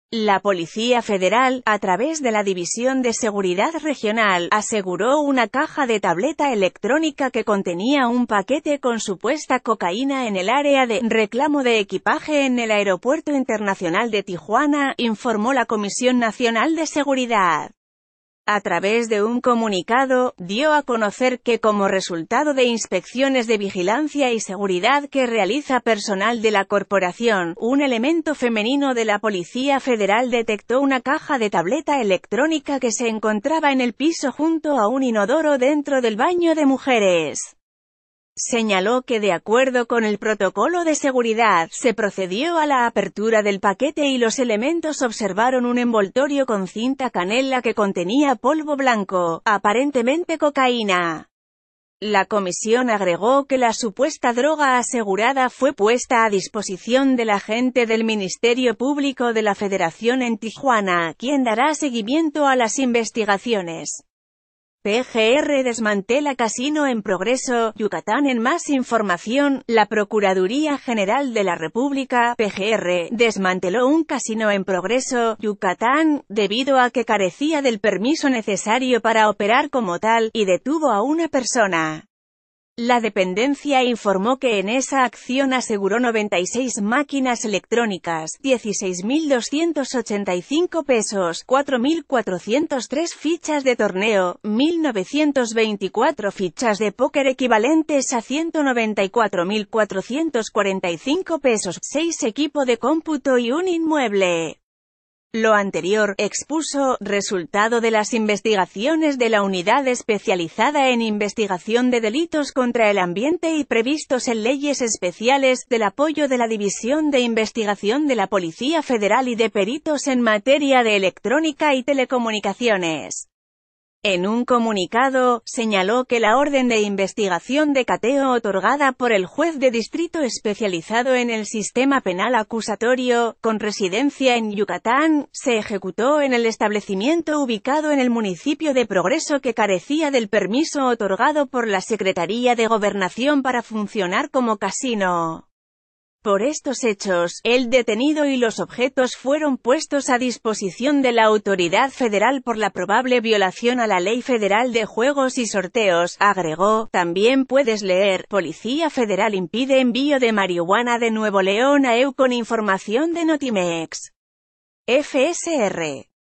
Aseguran paquete con cocaína en aeropuerto de Tijuana
La Policía Federal, a través de la División de Seguridad Regional, aseguró una caja de tableta electrónica que contenía un paquete con supuesta cocaína en el área de “reclamo de equipaje en el aeropuerto internacional de Tijuana”, informó la Comisión Nacional de Seguridad. A través de un comunicado,...